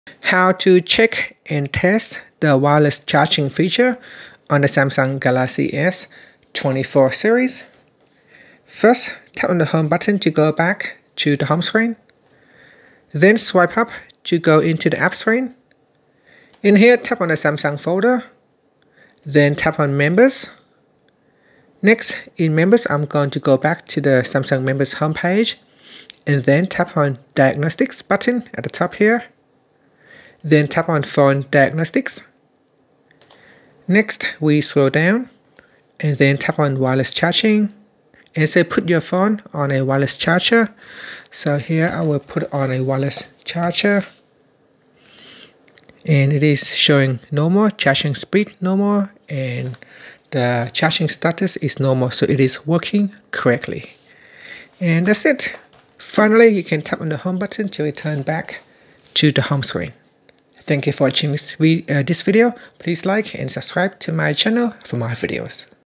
Galaxy S24/S24+/Ultra: How to Check and Test Wireless Charging
In this video, I will show you how to check and test the Wireless Charging on the Galaxy S24/S24+/S24 Ultra. Is your Samsung Galaxy S24's wireless charging not working as expected?  Don't worry! This video will show you how to diagnose and fix common wireless charging issues.